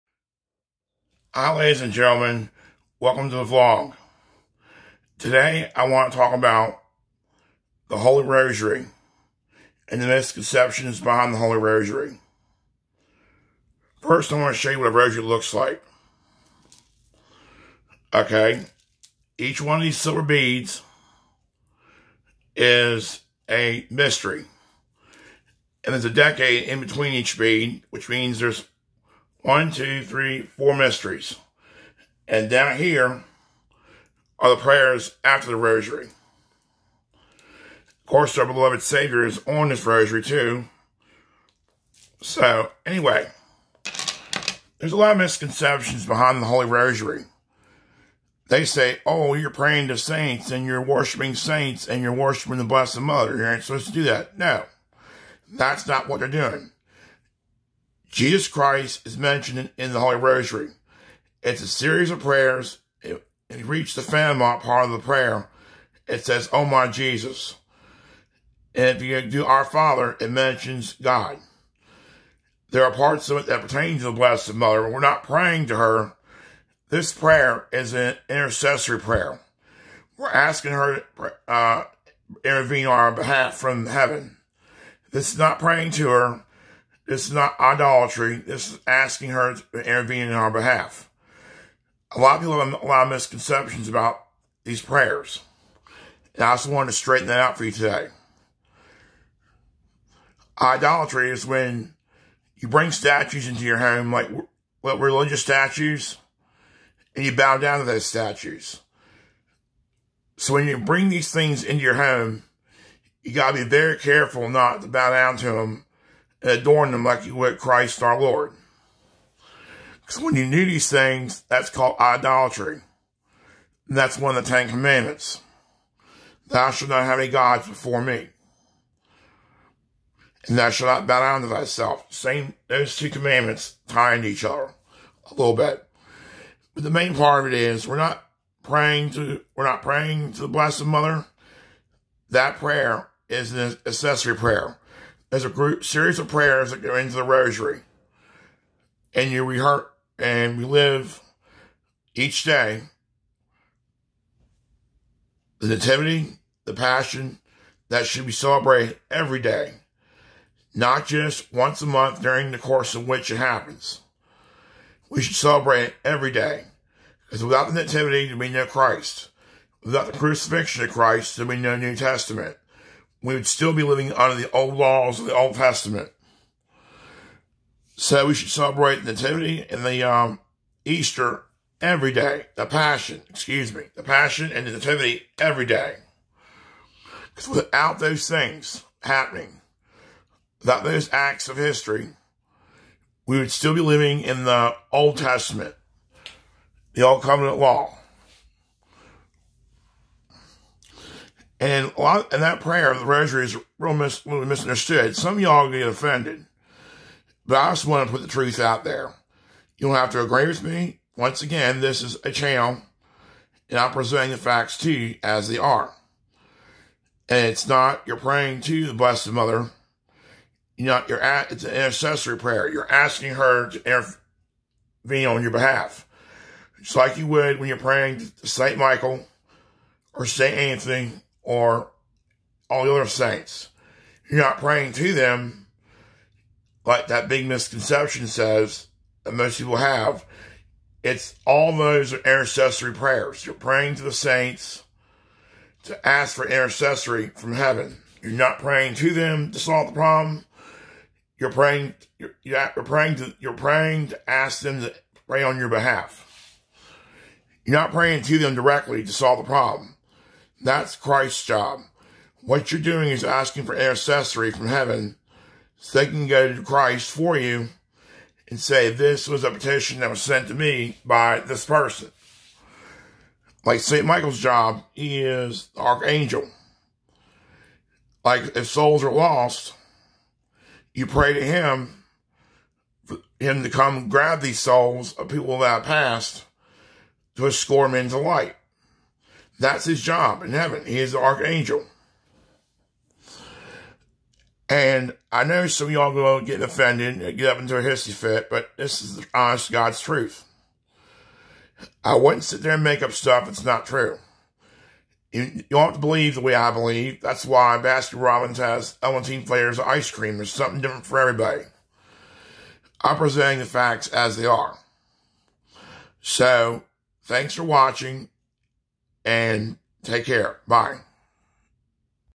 the Holy Rosary the the myths and truths.
today I talk about intercessory prayer such as the rosary and ancestry prayer from St Michael for example we don't pray to them we ask them to intervene on behalf but people think in other Christian branches that these prayers are wrong they have the misconception that you're praying to these people and adnoring them
for instance with a rosary Christ is mentioned in different part of the prayers that make up the Rosary.